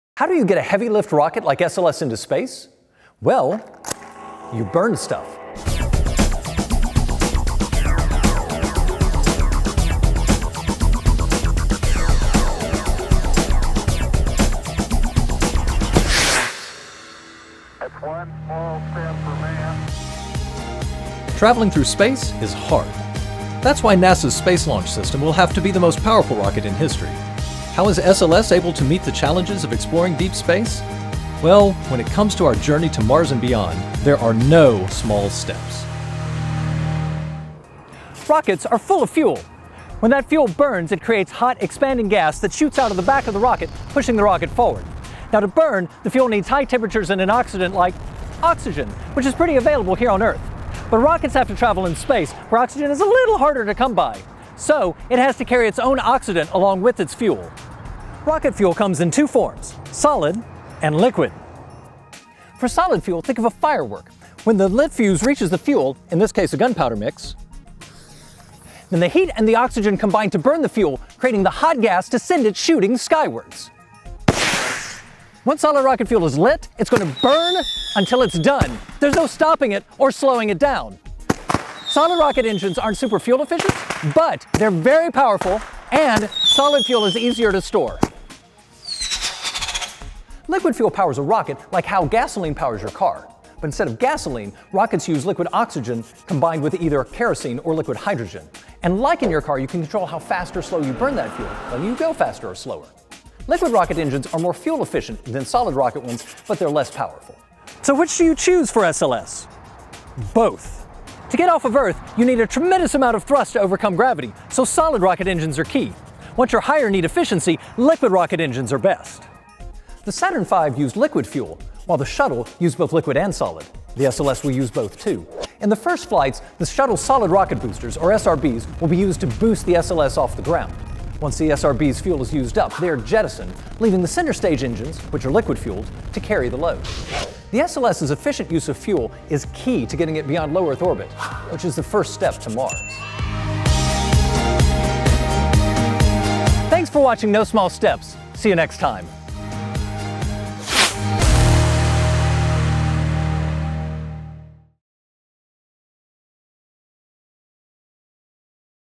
No Small Steps Episode 3: Rocket Fuel
The third installment of this video series takes a look at the basics of the monumental energy that makes NASA’s Space Launch System (SLS) rocket go up. The initial configuration of the SLS uses two different means of powering itself during launch – solid rocket boosters and liquid-fuel engines. Host Stephen Granade explores why we have boosters and engines, the difference between the two and what role each plays during a launch.